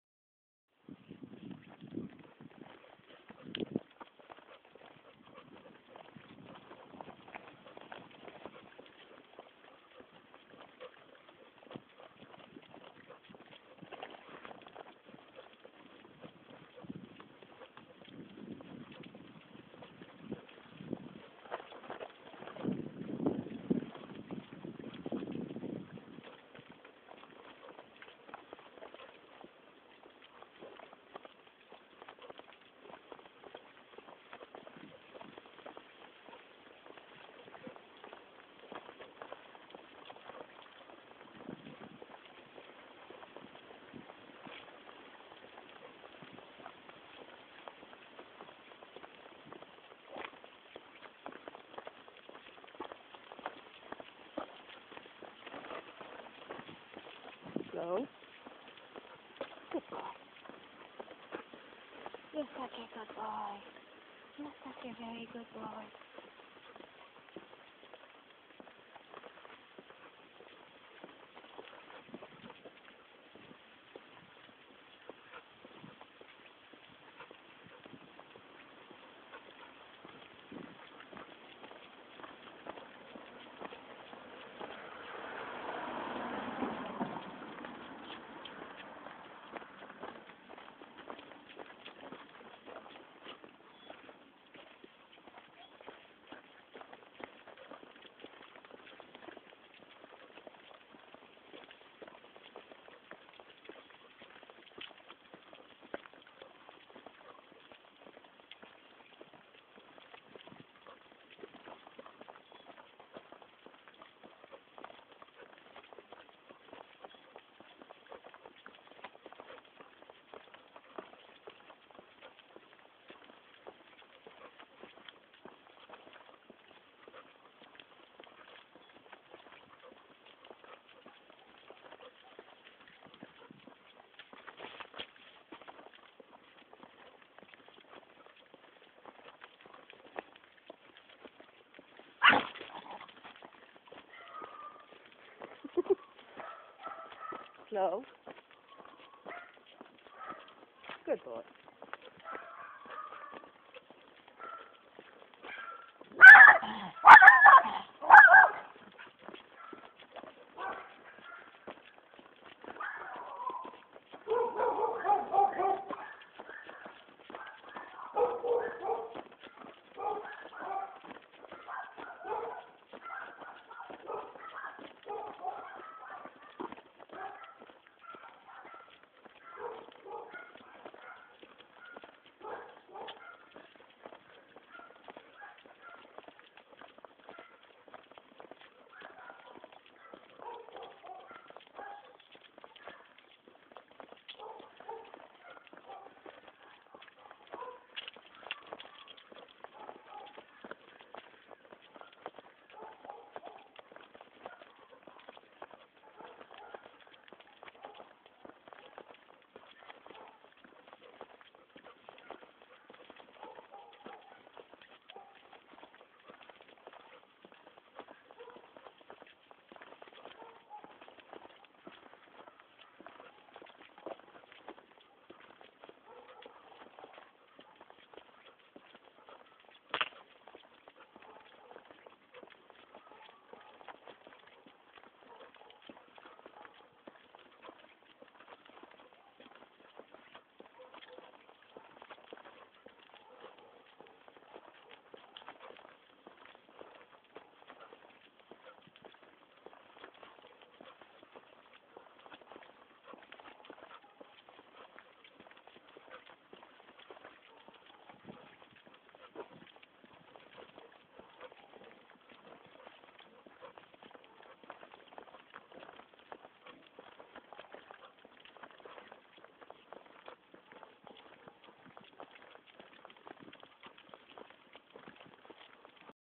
Goodman LLW
Goodman showing off his loose-leash walking skills (yes, he walks on my right side), even when being surprised by a dog barking from it's yard.
He also knows the "slow" cue to slow down while walking.
The leash is a hands-free leash.